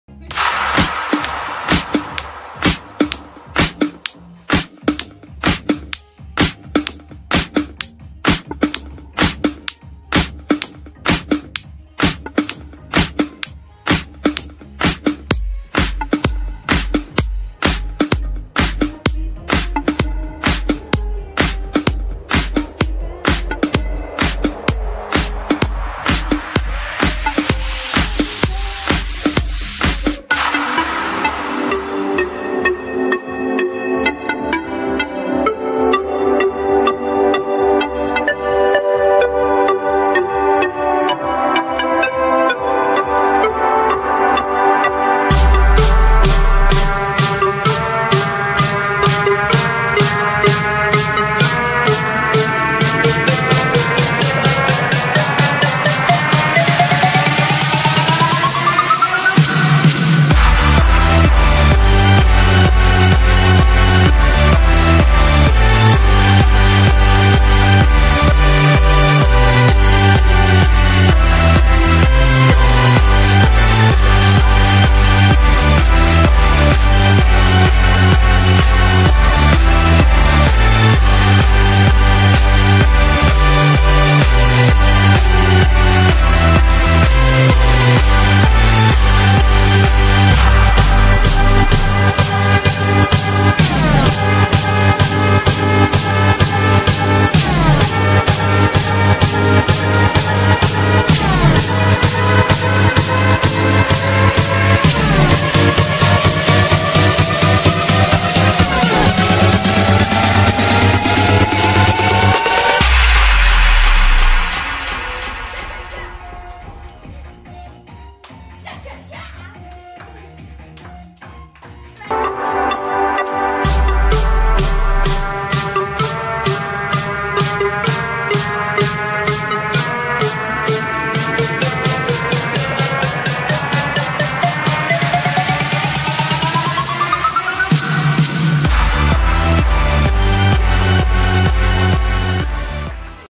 birthday edit
this is my birthday and I'm turning 12 so I made it a edit